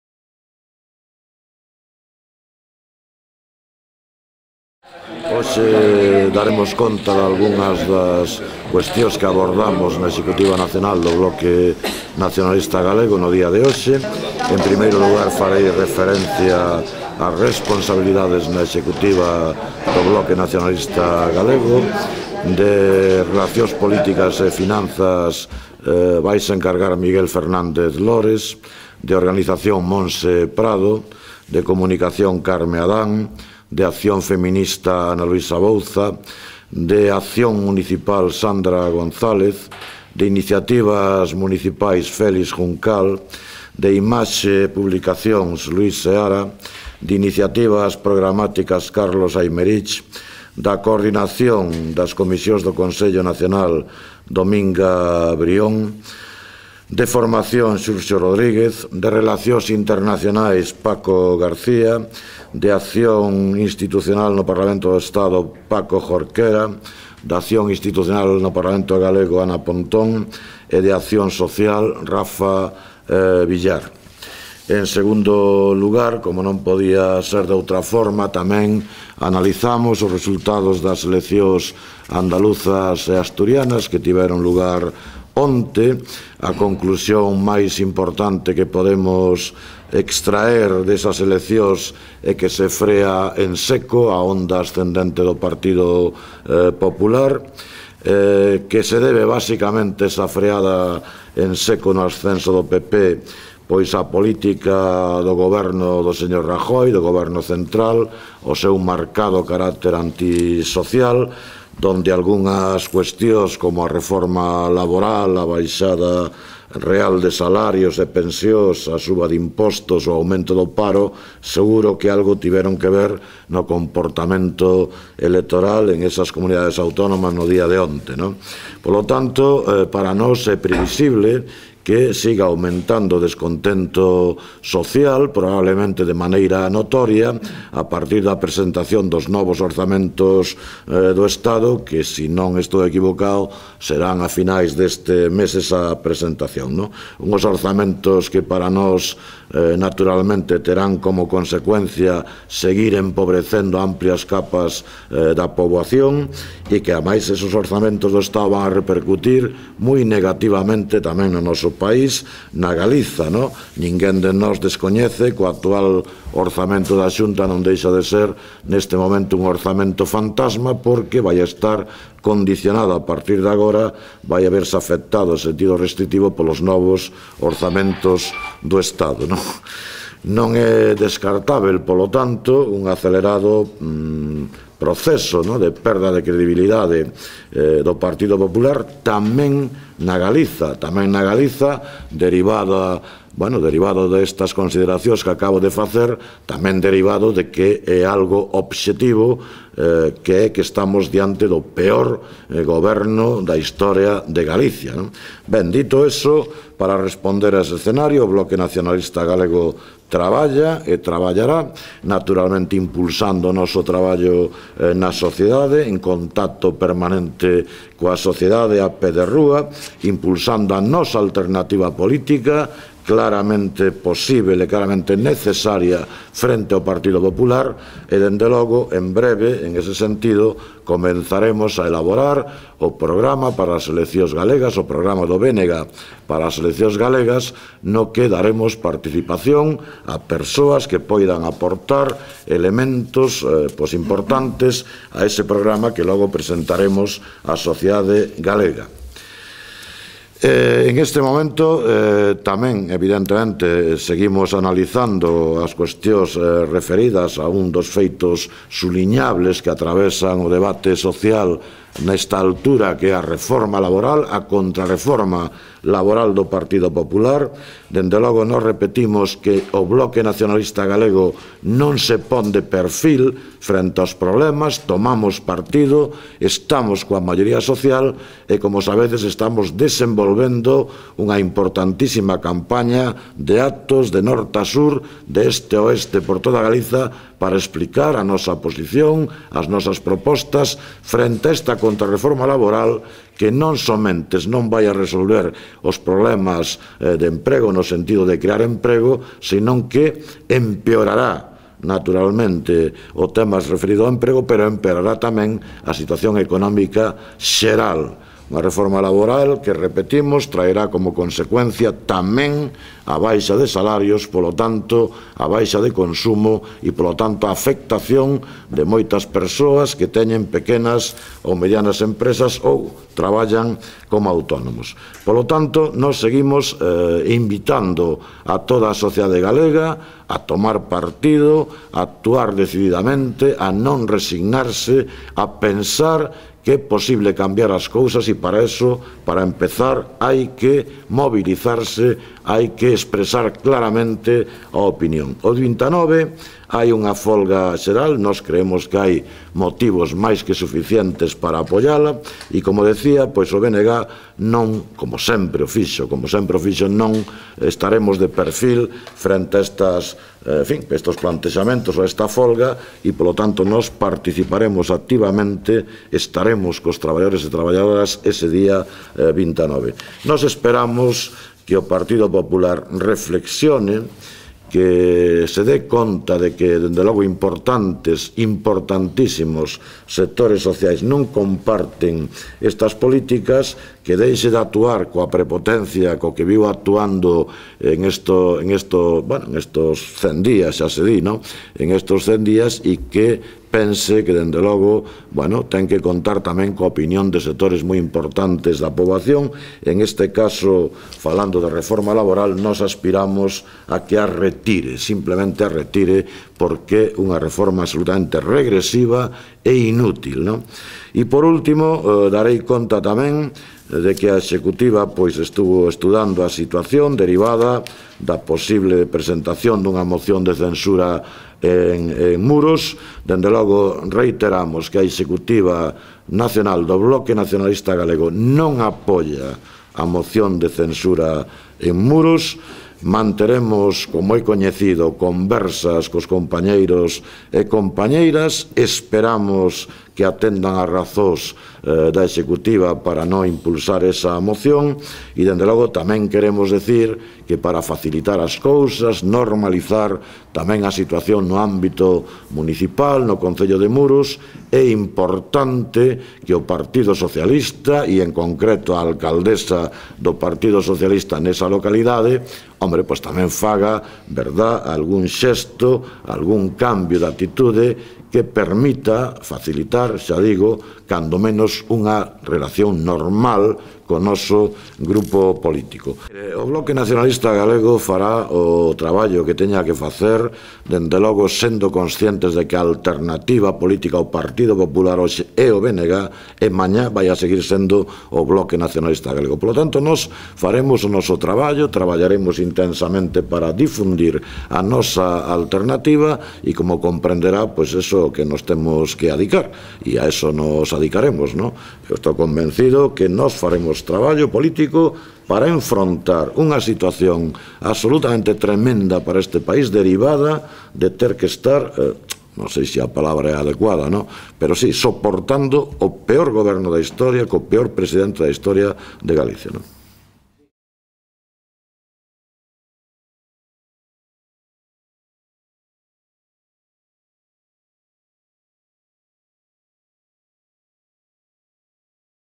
Guillerme: "Como en Andalucía e Asturias, o PP está a perder credibilidade en Galiza"
Subliña que "o BNG é hoxe a alternativa ao PP na Galiza, e tamén o será mañá". Valoración do portavoz nacional trala reunión da Executiva Nacional do BNG.